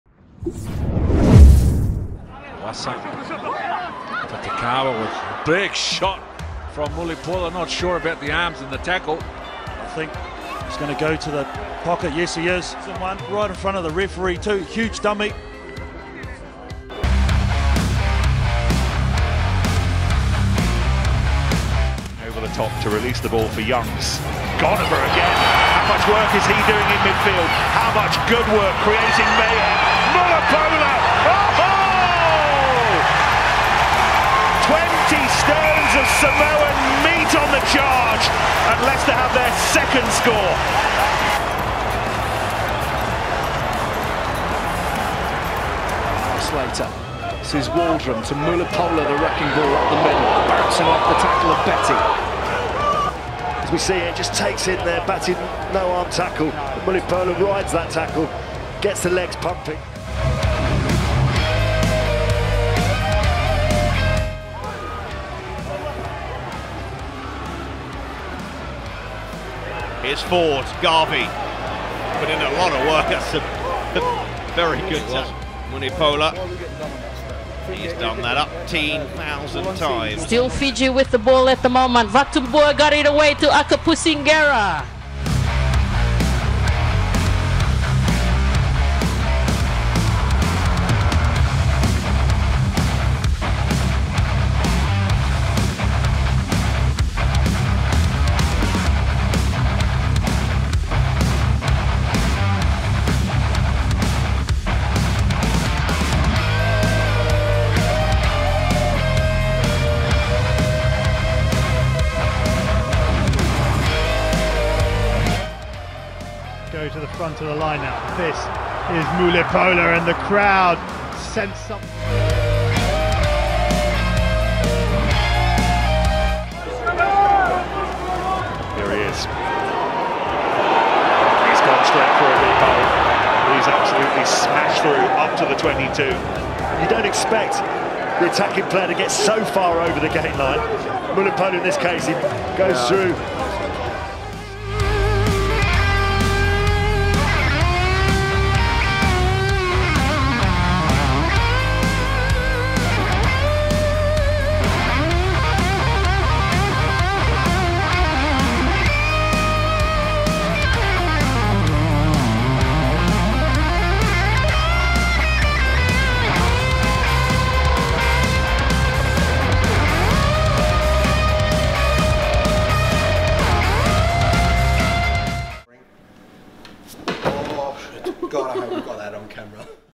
Mulipola destroying people for 3 minutes 13 seconds
Logovi'i Mulipola was one of the hardest rugby players to play the game. Sit back and enjoy some quality rugby content!